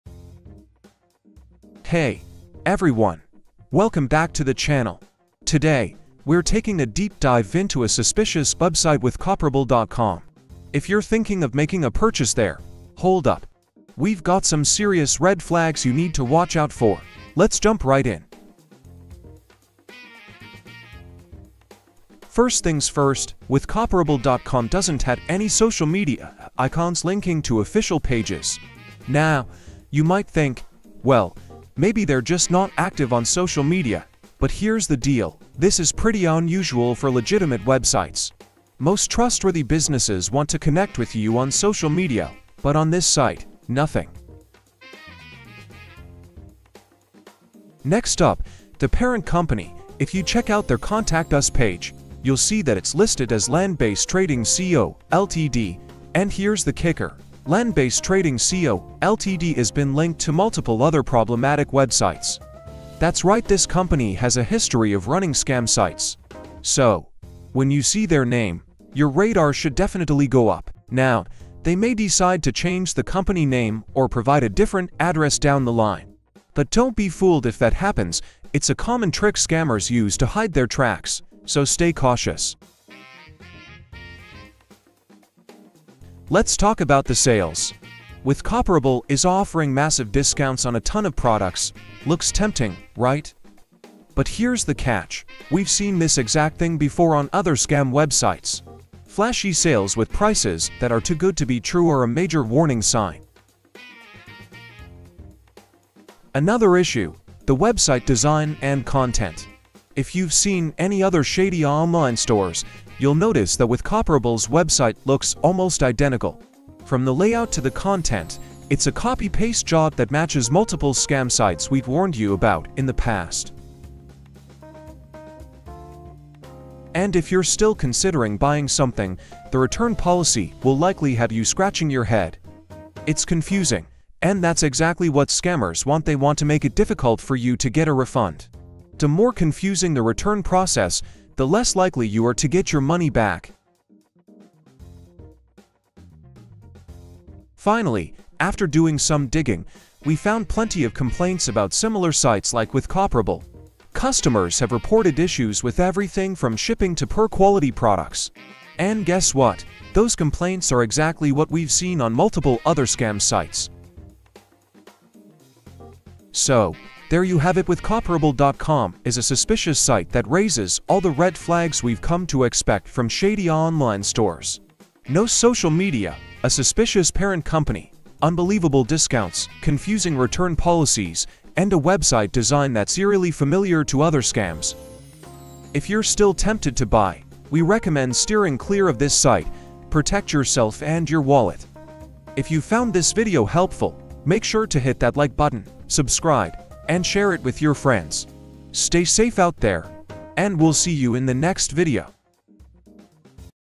Withcoparable.com Review: Is It a Scam or a Genuine Online Store?
Are you considering buying from Withcoparable.com? Before you make a purchase, watch out for these major red flags! In this comprehensive Withcoparable review, we’ll uncover the truth about whether this website is a genuine online store or a scam waiting to take your money.

From lack of social media icons to suspicious discounts and a confusing return policy, this site shows all the signs of a problematic online store. The parent company behind it, has a history of operating shady websites, which makes this review even more crucial for your safety.

Keep watching to learn more about the warning signs that should make you think twice before buying. Don’t get caught in the trap—Withcoparable.com may not be as trustworthy as it seems!